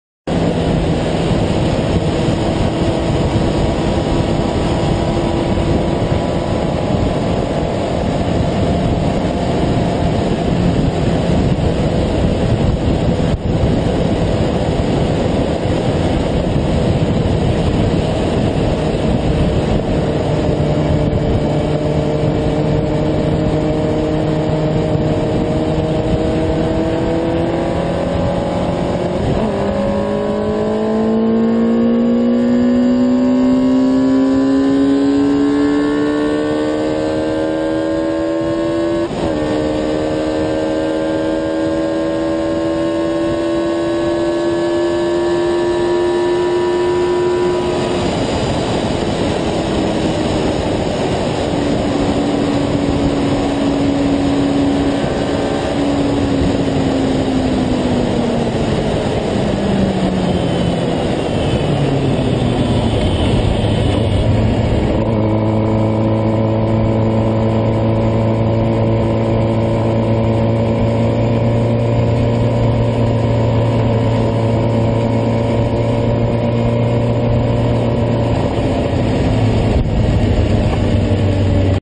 Kasksız 250 km hız yaptı
Motorsikletle kasksız 250 km hız yaparsak ne mi olur cevabı videoda